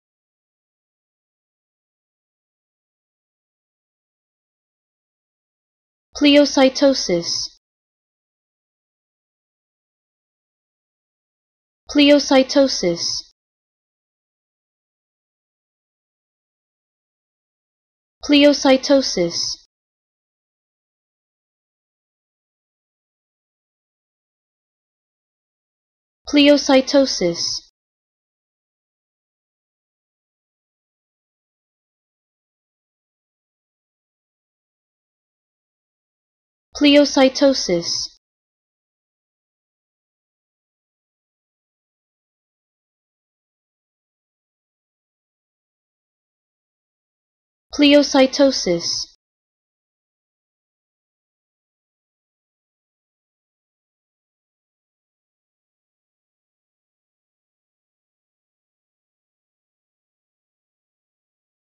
How To Say Pleocytosis
Learn how to say Pleocytosis with EmmaSaying free pronunciation tutorials.